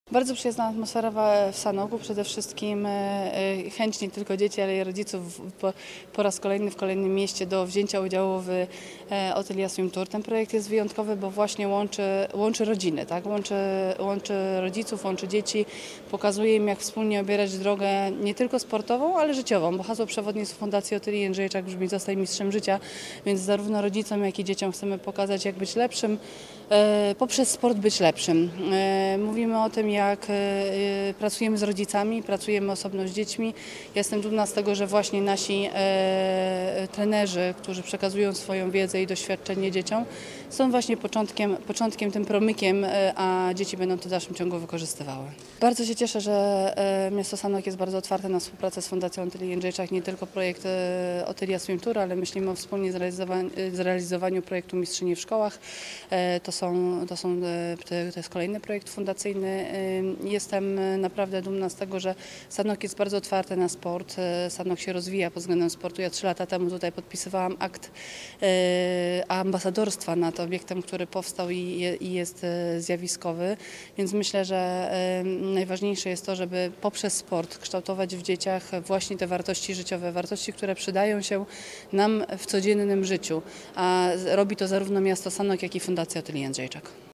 SANOK: Otylia Jędrzejczak szkoliła młodych adeptów pływania! (VIDEO, FOTO)
– Ten projekt jest wyjątkowy, bo łączy rodziny – mówi o akcji Otylia Swim Tour Otylia Jędrzejczak, która w sobotę przyjechała wraz ze swoim sztabem trenerskim do Sanoka, by przeszkolić młodych adeptów pływania i ich rodziców.

To trzecia odsłona tegorocznego Otylia Swim Tour. Po wizytach w Lublinie i Nowym Targu tym razem trenerzy Fundacji Otylii Jędrzejczak ćwiczyli z dziećmi w Sanoku.
– Cieszę się, że Sanok jest otwarty na współpracę. Myślimy nie tylko o Otylia Swim Tour, ale również o zrealizowaniu projektu Mistrzynie w Szkołach. Najważniejsze jest to, żeby poprzez sport kształtować u dzieci wartości, które potrzebne są w życiu. A robi to zarówno miasto Sanok, jak i Fundacja Otylii Jędrzejczak – mówi Otylia Jędrzejczak.

Burmistrz Sanoka Tomasz Matuszewski cieszy się ze współpracy z fundacją najlepszej polskiej pływaczki. – Z Otylią poznaliśmy się trzy lata temu, z mojej inicjatywy została ambasadorem naszych basenów. Cieszę się, że jej fundacją wróciła do Sanoka i chce z nami współpracować dla dobra młodzieży – mówi burmistrz Sanoka.

W trakcie całego dnia dzieci otrzymały kompleksową wiedzę zarówno od trenerów przygotowania motorycznego w hali, jak i trenerów pływania w wodzie. W sumie, z przerwą na obiad, dzieci spędziły w sobotę blisko osiem godzin na aktywności sportowej. Otrzymały też atrakcyjne pakiety startowe ufundowane przez parterów i sponsorów Fundacji Otylii Jędrzejczak, m.in. firm Wiśniowski, Jurajska i HMS.

– W Sanoku jest bardzo przyjazna atmosfera, a przede wszystkim chęć nie tylko dzieci i rodziców do wzięcia udziału w Otylia Swim Tour. Ten projekt jest wyjątkowy, bo łączy rodziny. Zarówno rodzicom, jak i dzieciom chcemy pokazać, jak poprzez sport być lepszym. Jestem dumna, że nasi trenerzy są promykiem, a dzieci będą to wykorzystywały – dodaje Jędrzejczak.

Mimo wysokich temperatur dzieciaki ćwiczyły – zarówno w wodzie, jak i w hali sportowej – z wielką ambicją. – Uwielbiam pływać żabką, bo to pomaga mi pozbyć się stresu i czuję się lepiej. Trenuję raz w tygodniu, a w sumie pływam już pięć lat. Cieszę się, że mogę spotkać się z taką mistrzynią – mówi Brygida z Sanoka.

Jej kolega Michał liczy, że dzięki wskazówkom Otylii Jędrzejczak i jej trenerom poprawi technikę pływania. – Najbardziej w stylu motylkowym, który jest moim ulubionym. Bardzo lubię pływać, a najfajniejsza jest rywalizacja i przyjemność z niej wynikająca – dodaje.

W czasie, gdzie dzieci ćwiczyły pod okiem jedenastu trenerów pływania i przygotowania fizycznego, rodzice brali udział w wykładach psycholog sportowej Beaty Mieńkowskiej i dietetyk sportowej Agaty Grębskiej. – Porozmawiamy o wskazówkach, jak zdrowo się odżywiać. Chciałabym dać jak najwięcej praktycznych wskazówek, jak wykorzystać to w kuchni i przygotowywać odpowiednie posiłki – mówiła Grębska tuż przed wykładem.

Projekt Otylia Swim Tour finansowany jest przez Ministerstwo Sportu i Turystyki, a impreza w Sanoku współfinansowana jest ze środków Miasta Sanok. Na tegorocznej mapie Otylia Swim Tour jest pięć miast.